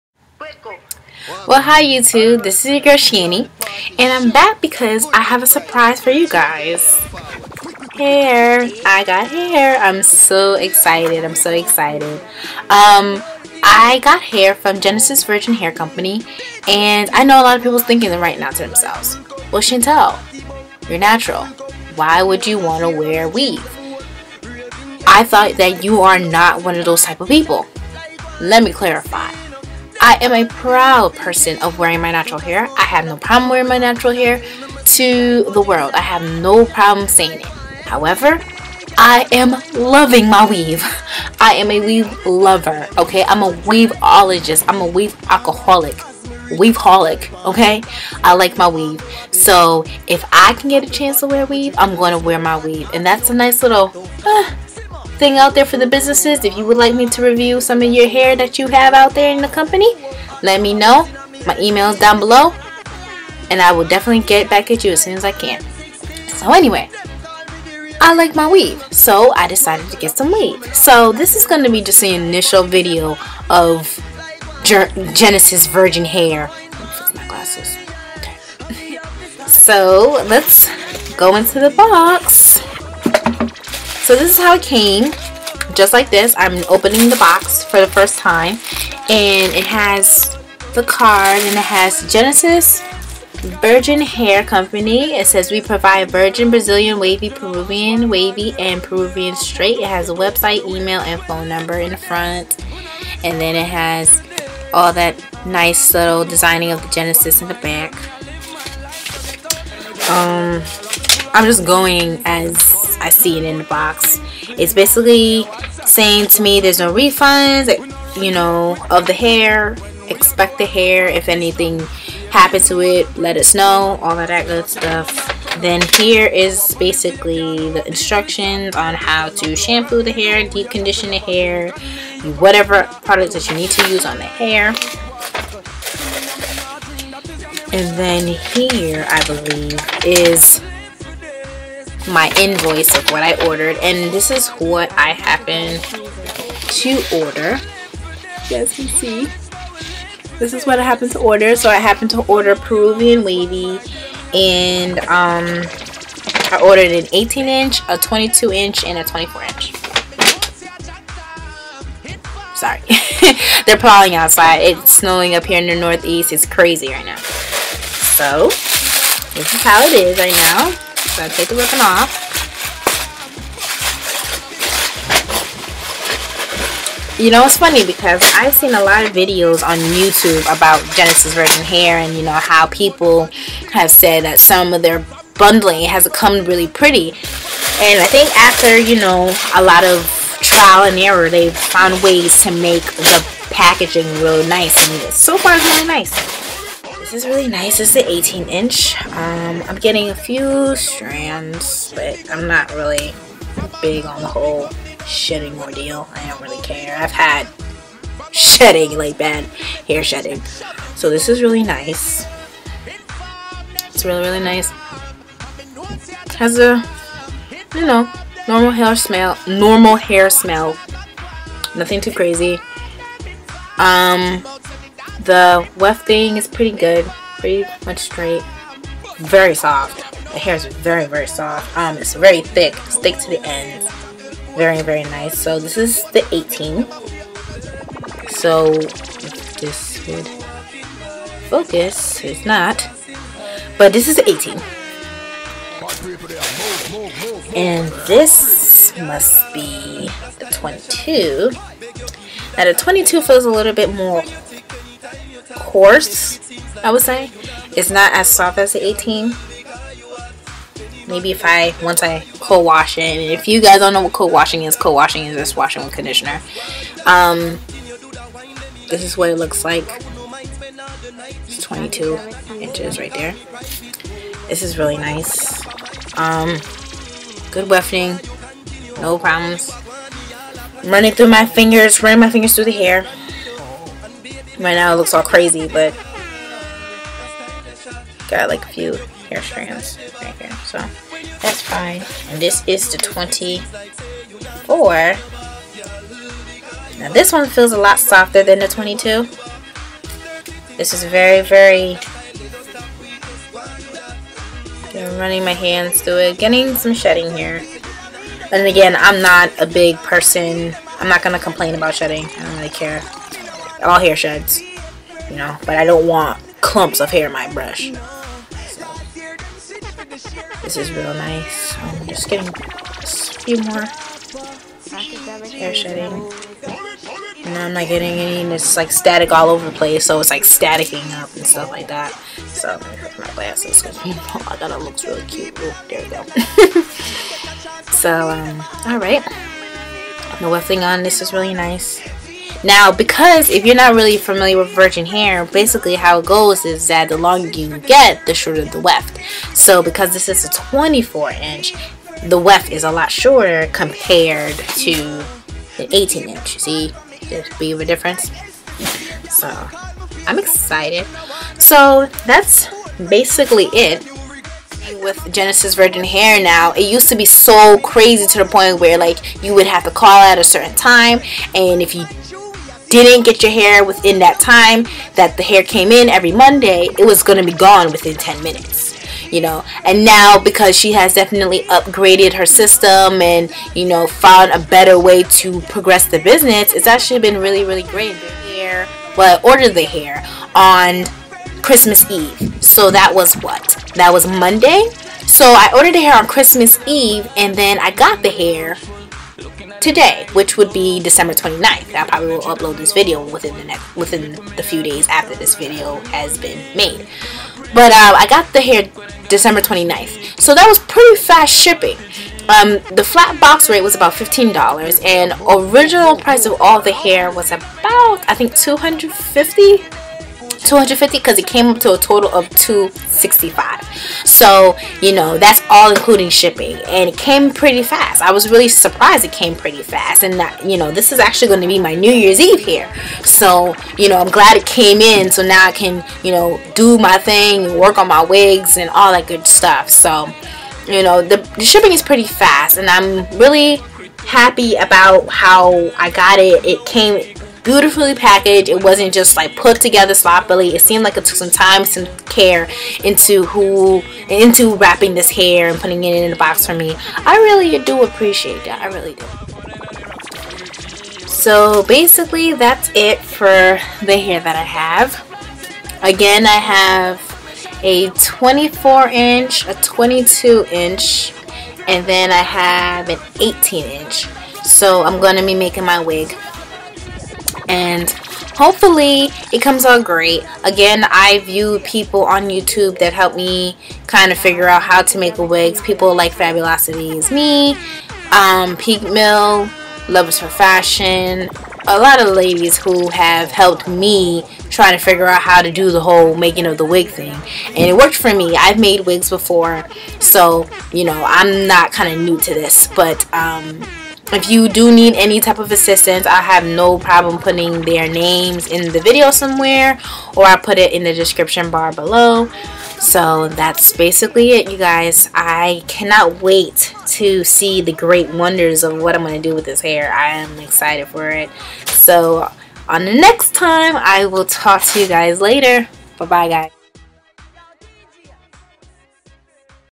Genesis Virgin Hair Peruvian Wavy.. Initial Review
OPEEEENN MEEEE!!!
ABSOLUTELY LOVE THE HAIR I BOUGHT... I will make a wig and a vid of how the hair is doing.  Toodles

Shanny♡♡♡

♡ I n s t a g r a m - Shantedion
♡T w i t t e r - Shantedion